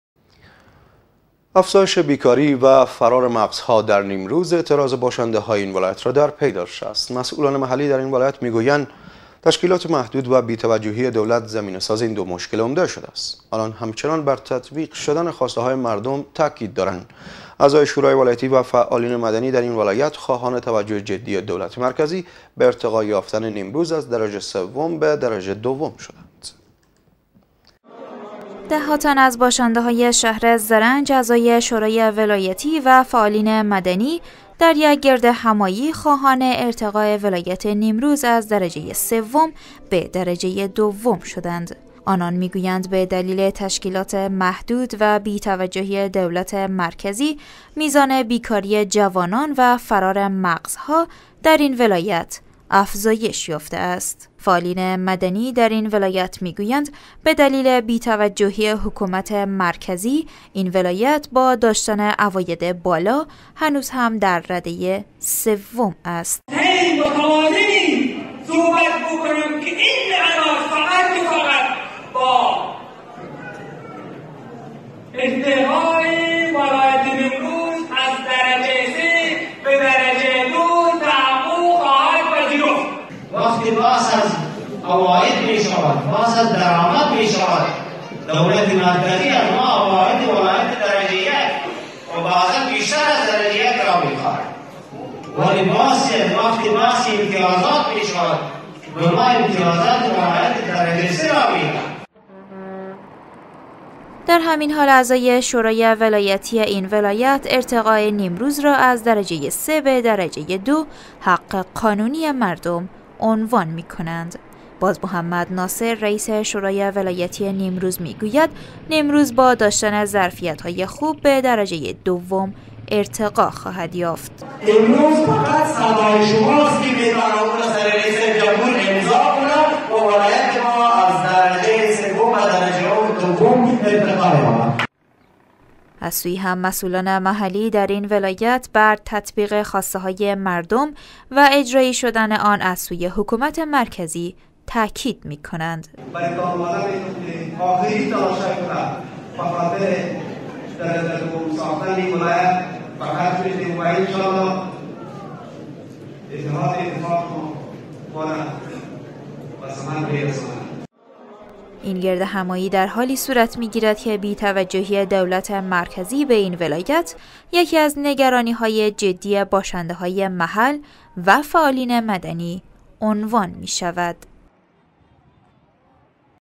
اعتراض باشندگان نیمروز به بی توجه ای حکومت مرکزی
جوانان تحصیل کرده در نیمروز یا بیکارند و یا هم به خارج از کشور رفته اند